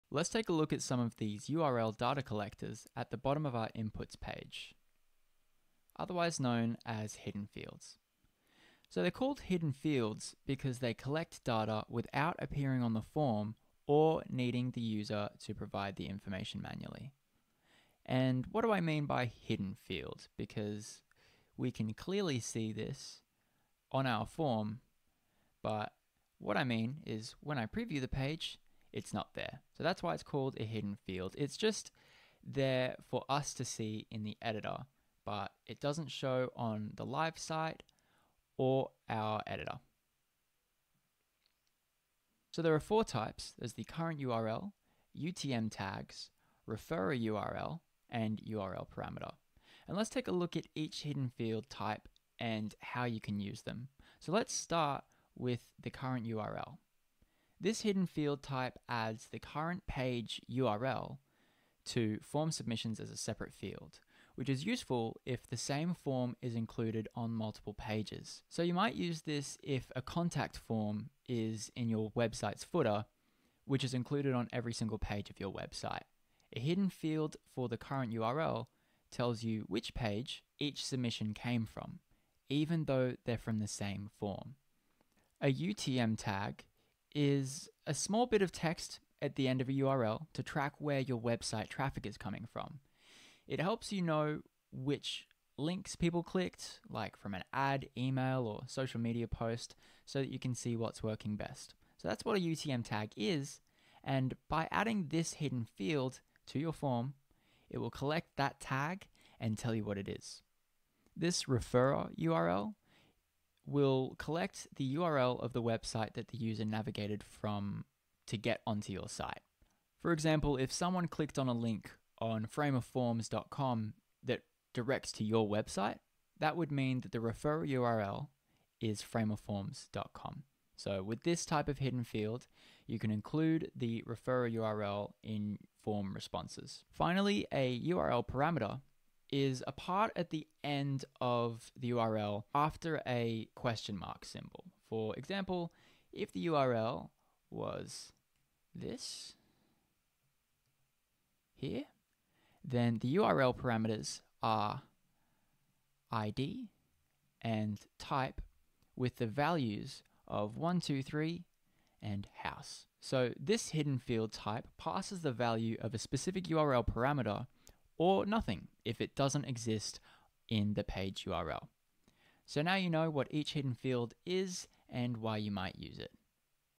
Adding Hidden Fields to your Framer Form (FramerForms Plugin)
In this video, Wes shares what each URL data collector, aka hidden field is, and when to use them.

Want more Framer plugins and products? 👉 insertframe.io